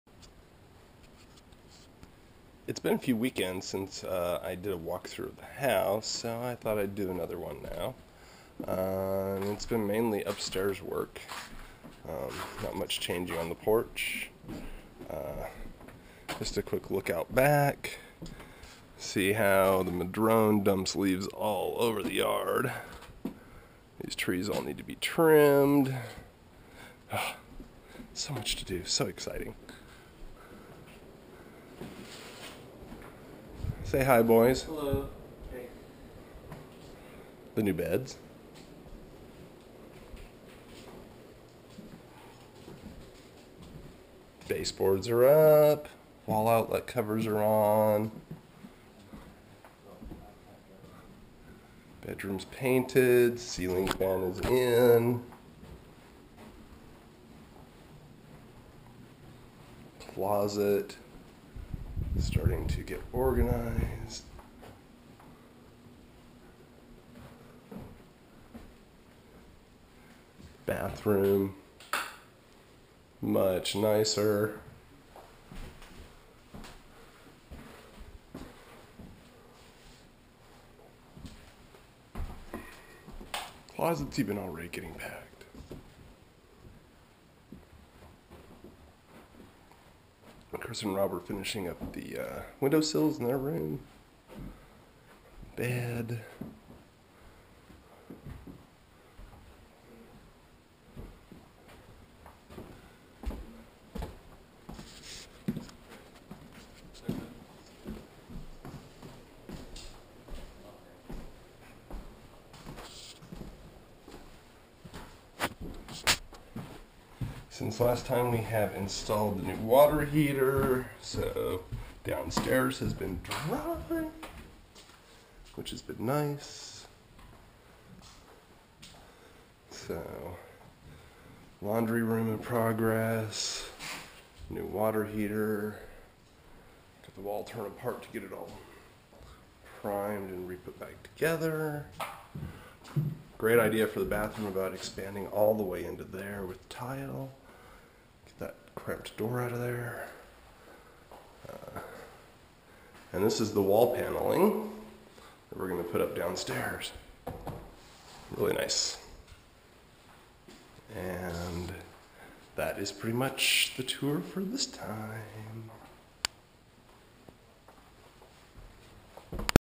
Pioneer House Walkthrough - 08-24-2011
Mostly finished with the upstairs. Carpet, baseboards, outlets, all the details have been taken care of!